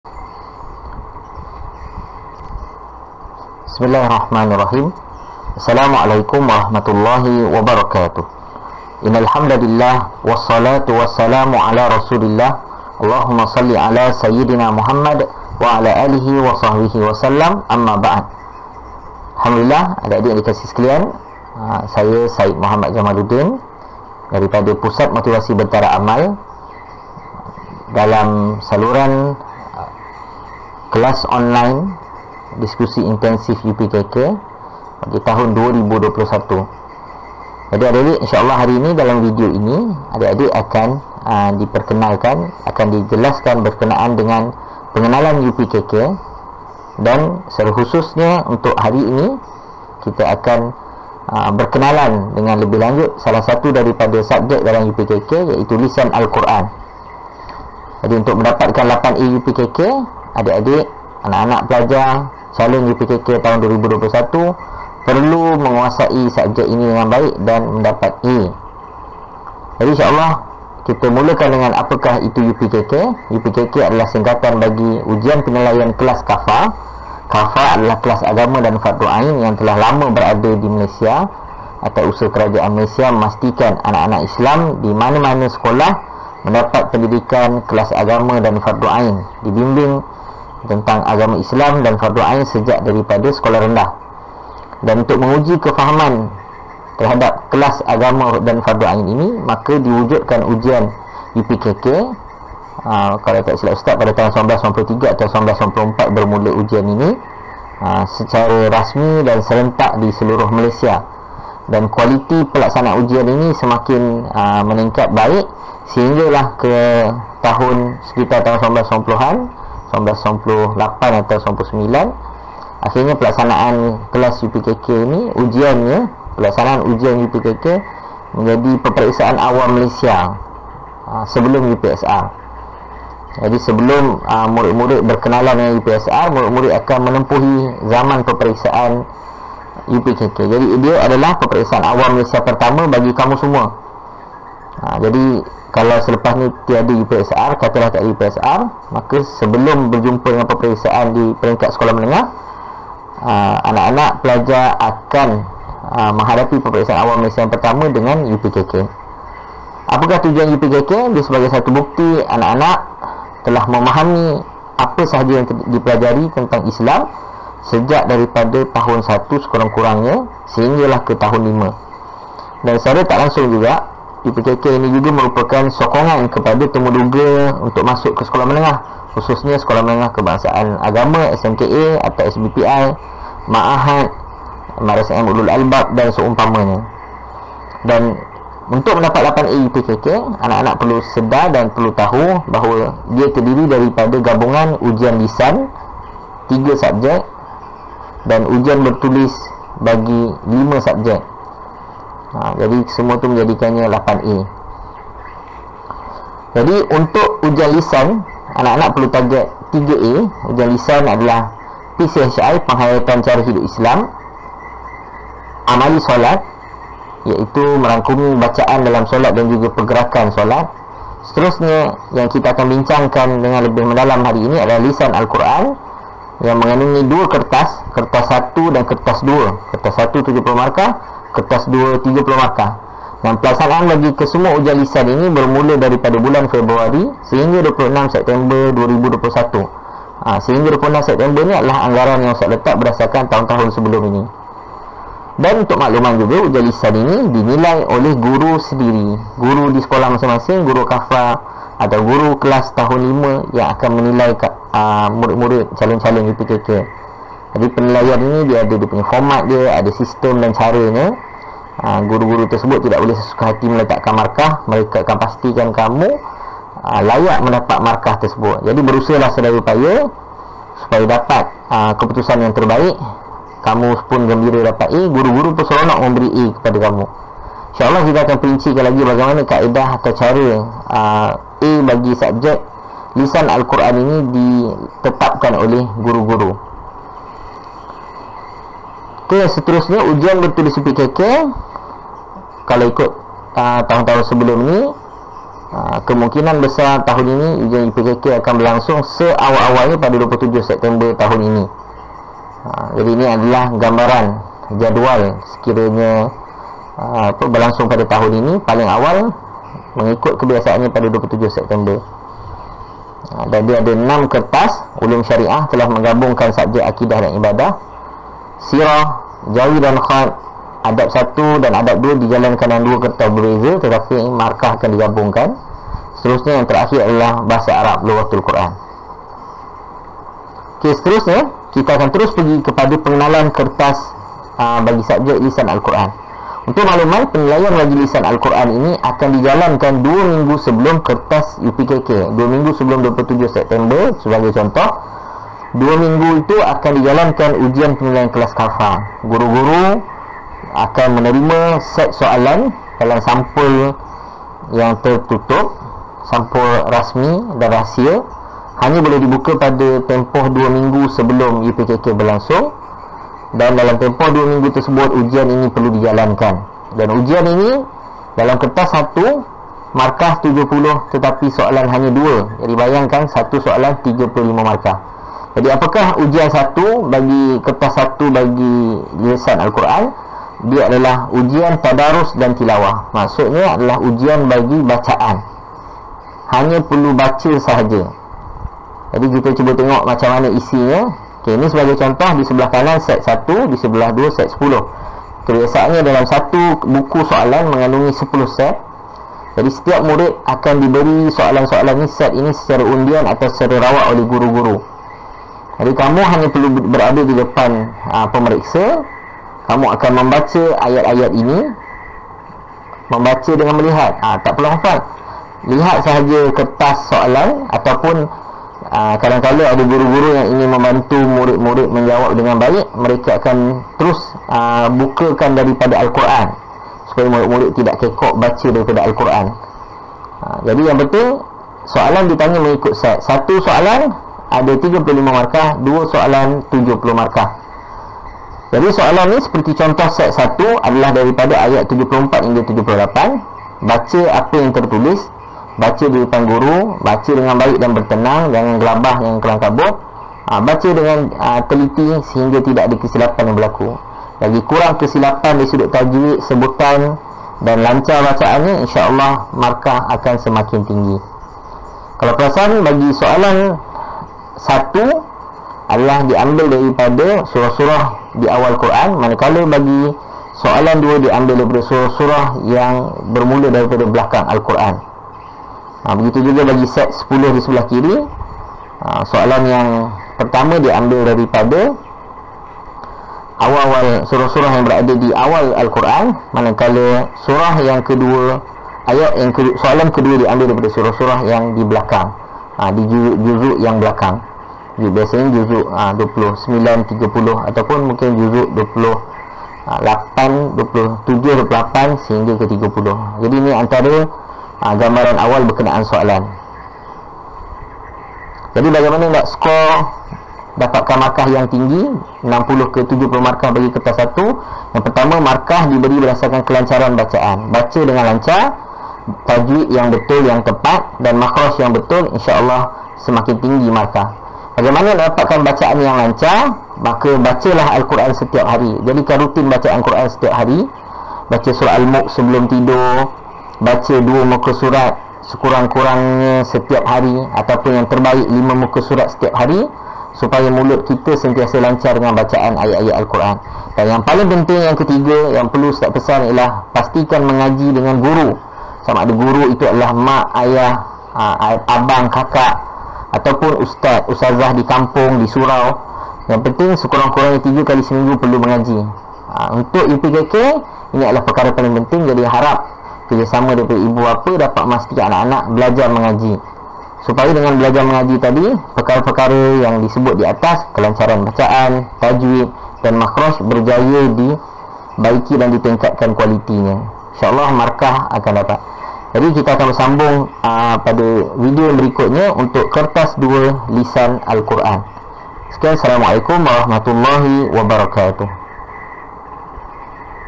002 - Lisan AQ UPKK Kertas 1
Video ini memaparkan penerangan ringkas berkaitan UPKK dan pengenalan berkenaan salah satu subjek yang dinilai dalam UPKK iaitu subjek Lisan al-Qur'an. Video ini akan menjelaskan maklumat penting berkaitan Kertas 1 Lisan al-Qur'an ( 70 markah & 2 soalan )